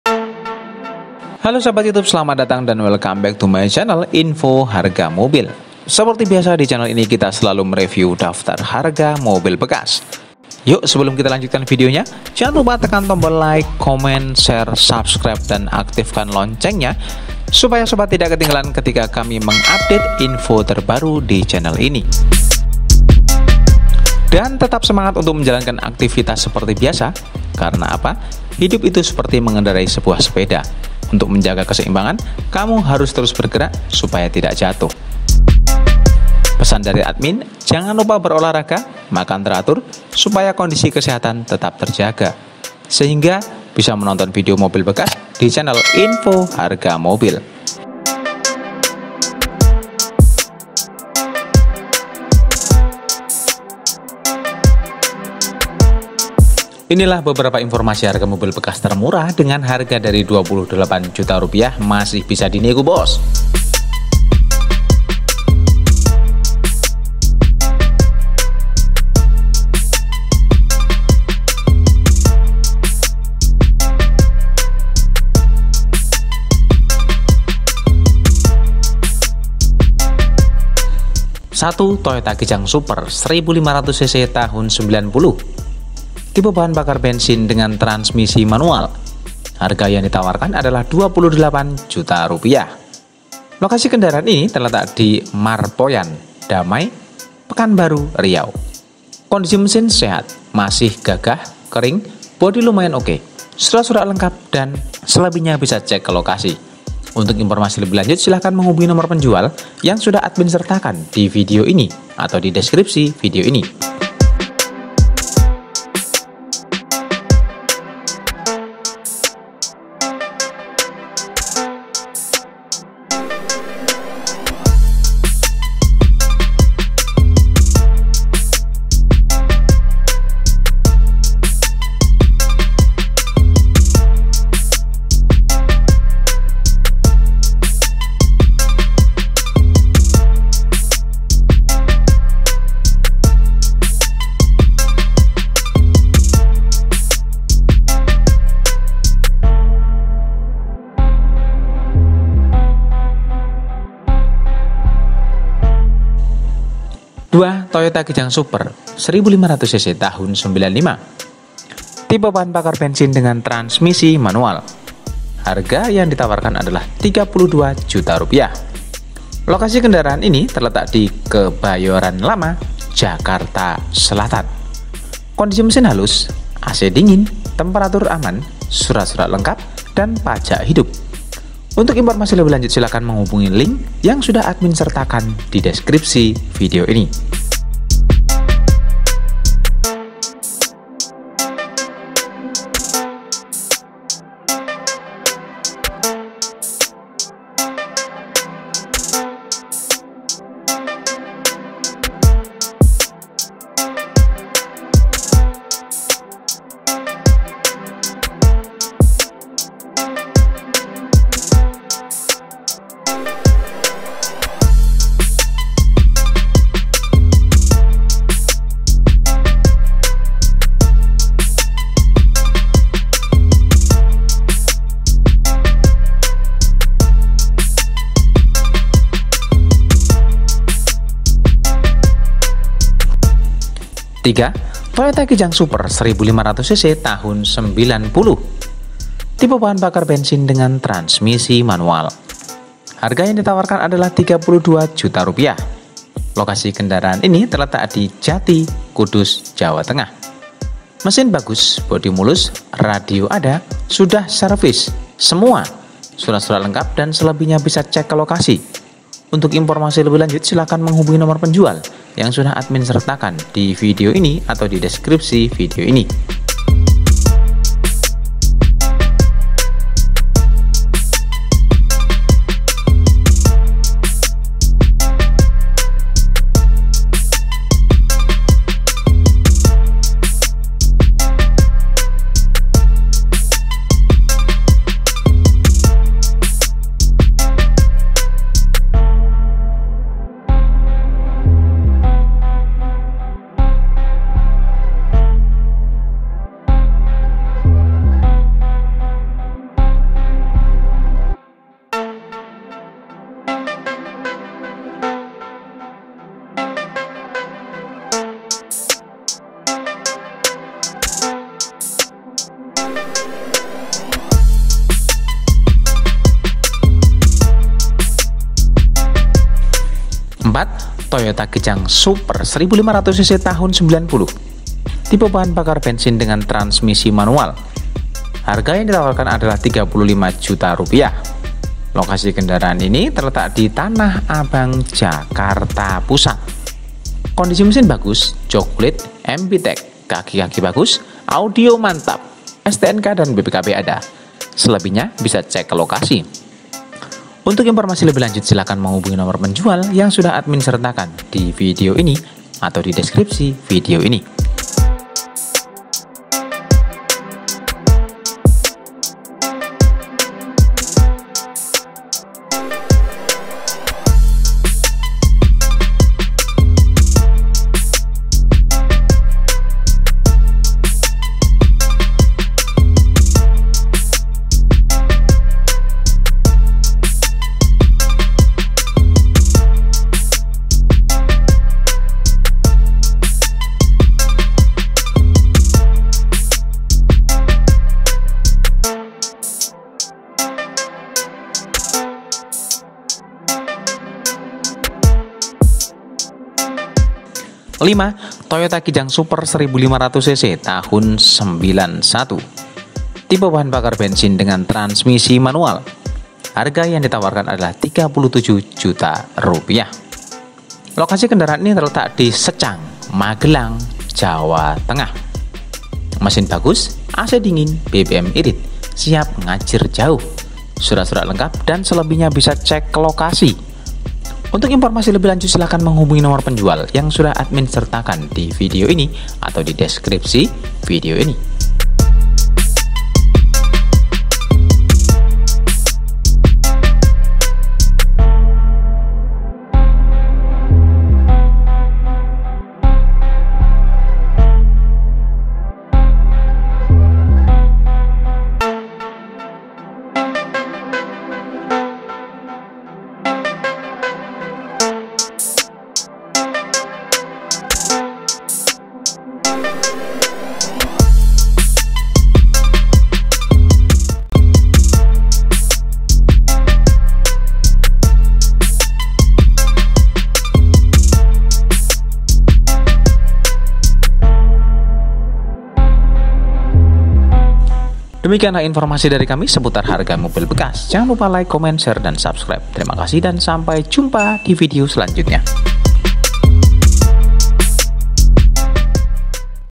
Sudah Murah Toyota Kijang Super Harga Termurah Hanya 28 Juta
SELAMAT DATANG, ATAS KUNJUNGANNYA DI CHANEL INI. MARI KITA SIMAK REVIEW DAFTAR HARGA HARGA MOBIL BEKAS

1. TOYOTA KIJANG SUPER 1500CC TH 90

2. TOYOTA KIJANG SUPER 1500CC TH 95
HP : -

3. TOYOTA KIJANG SUPER 1500CC TH 90

4. TOYOTA KIJANG SUPER 1500CC TH 90

5. TOYOTA KIJANG SUPER 1500CC TH 91